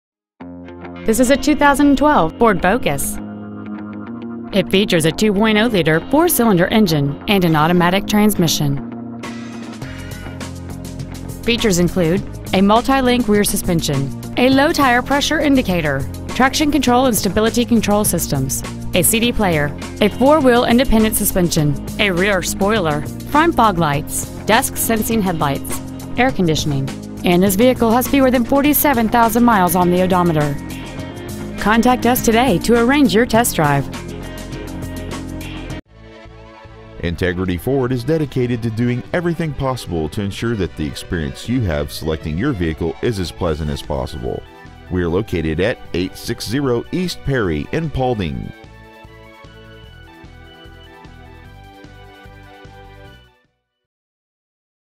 2012 Ford Focus Paulding OH
We have been honored to serve the Paulding OH area , we promise that your experience at our dealership will exceed your expectations !

 Year : 2012
 Make : Ford
 Model : Focus
 Engine : 2.0L I4 16V GDI DOHC FLEXIBLE FUEL
 Trans . : AUTOMATIC
 Exterior : INGOT SILVER METALLIC
 Miles : 46,851
 Interior : CHARCOAL BLACK
 Stock : L01957

 860 E Perry
 Paulding , OH 45879